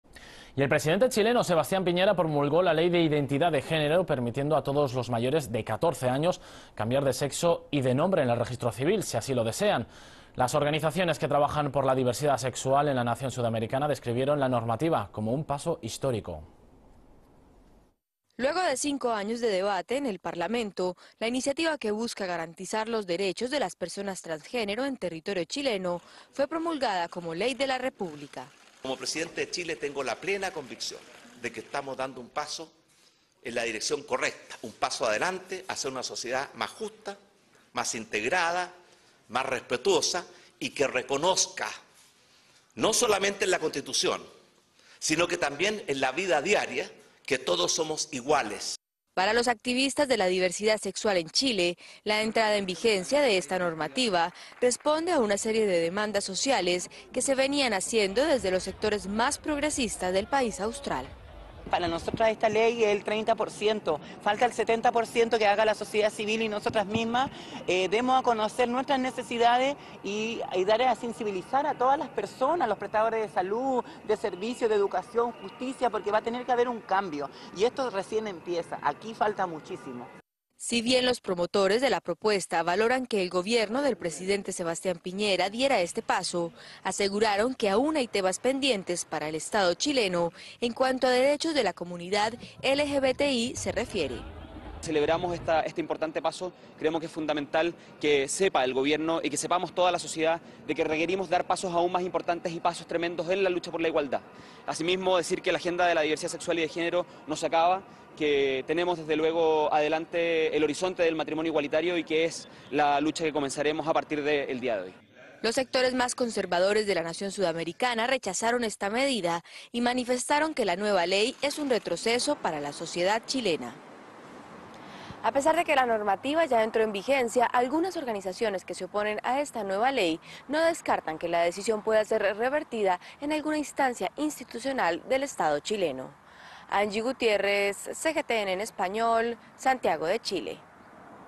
El presidente Sebastián Piñera promulga la ley que permite el cambio de sexo en el registro civil
El presidente chileno, Sebastián Piñera, promulgó la ley de identidad de género, permitiendo a todos los mayores de 14 años cambiar de sexo y de nombre en el registro civil, si así lo desean. Las organizaciones que trabajan por la diversidad sexual en la nación sudamericana describieron la normativa como un paso histórico.